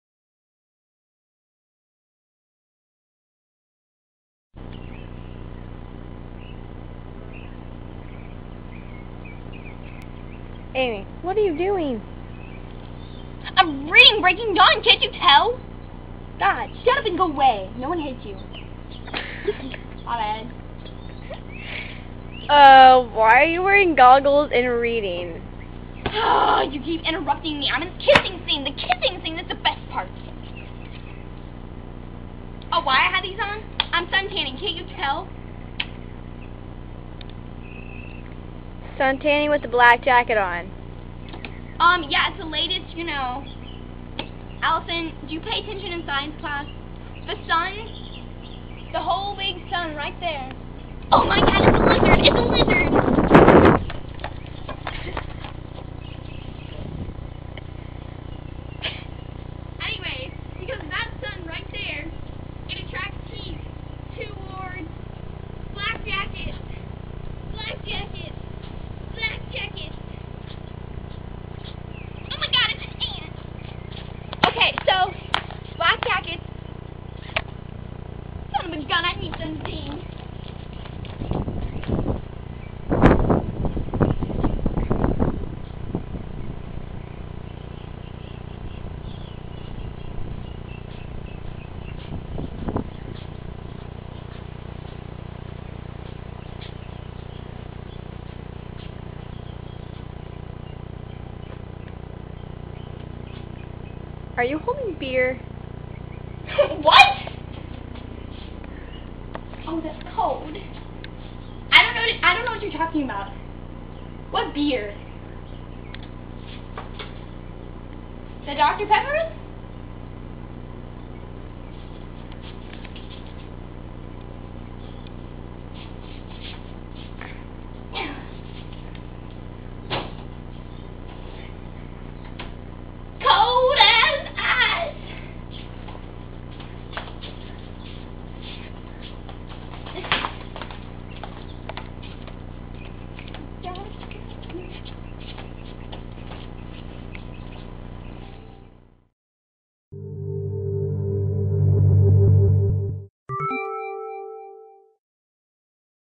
Goggles
im suntanning in goggles and a hoodie :)  ... wow im smart